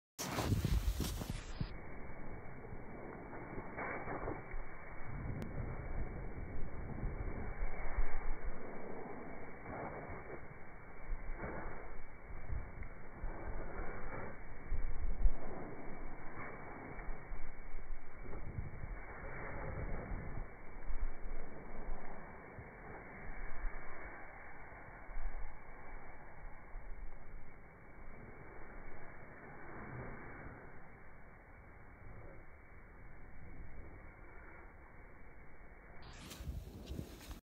Winterwandern im Karwendel
Die Schneekristalle wirbeln durch die Luft, die Sonne scheint, die Berge strahlen im frischen Weiß - meine schönsten Winterwanderungen im Karwendel